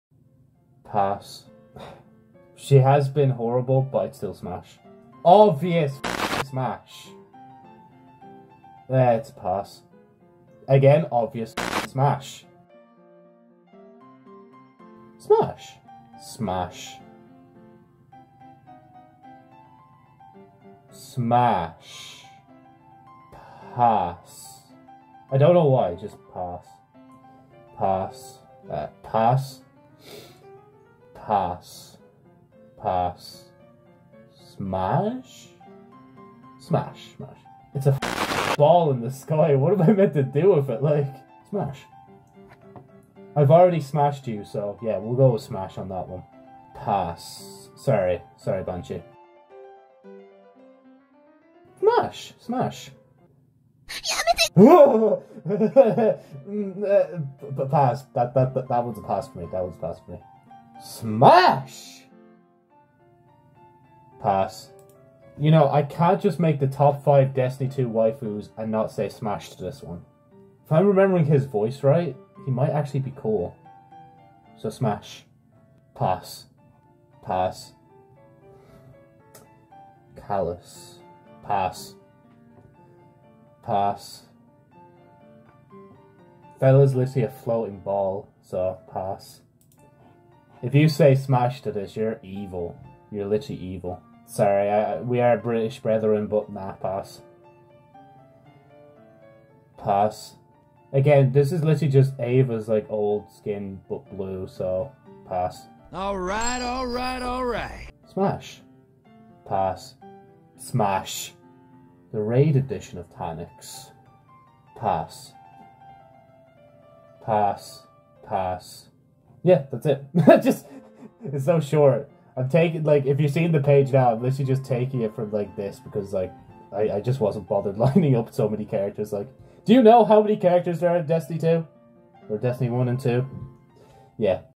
Smash or Pass (Destiny Edition)
Welcome to another weird memey kinda video. I unironically had a lot of fun making this over the thought of making other Destiny video ideas that I have stored in my cranium so... yeah, expect more of this for a bit before I inevitably either dip for another long time or upload actually destiny 2 commentary/news videos!

Also, I know I didn't include all the characters but I didn't want to spend ages on this so I only took the list google gave me when I searched up Destiny characters.

★ Featured: Destiny 1 and 2 Characters/NPCs